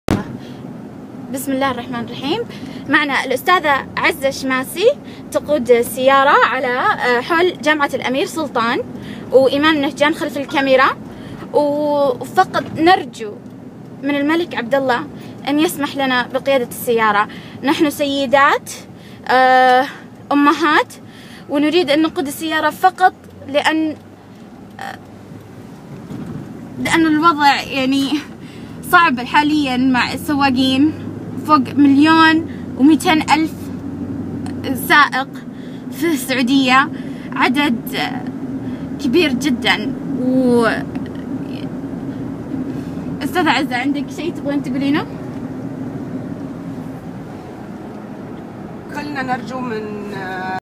Driving in Saudi with Saudiwoman
Mrs. Azza Shamasi driving in Riyadh with Eman Al Nafjan filming.